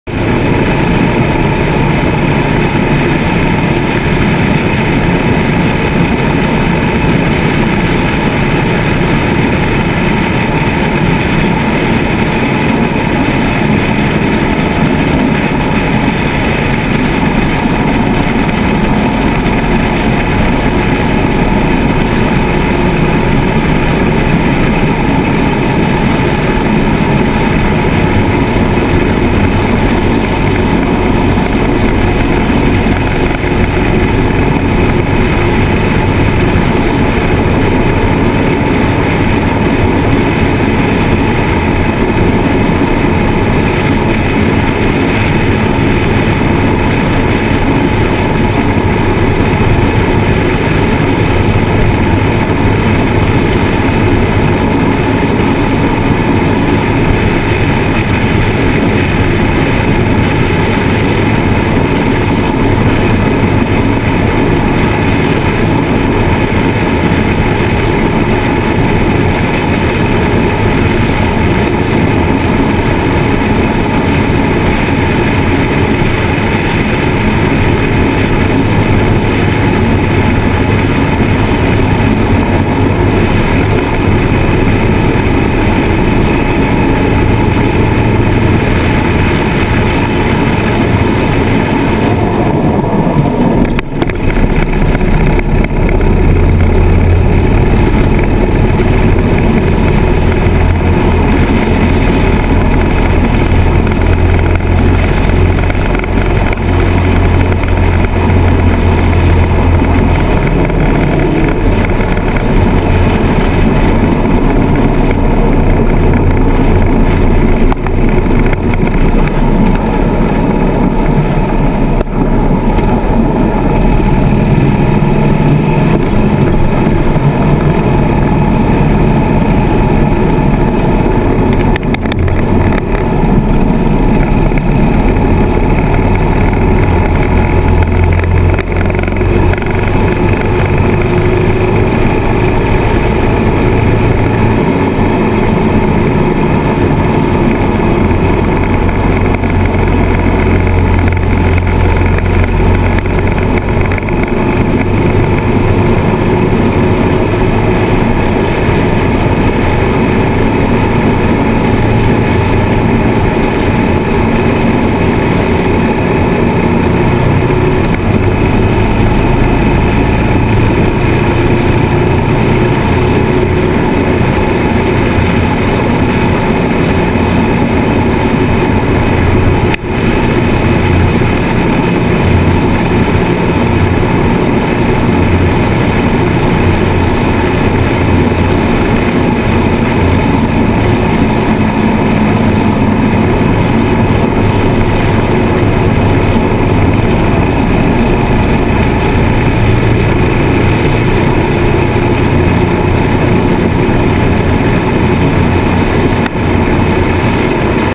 Oh Deer... Headed toward Susansville
Riding Boot's Sportster along Eagle Lake, headed toward Susansville. about 1:30 into the video you will see the bike nosedive and hear the brakes, then you will see the deer!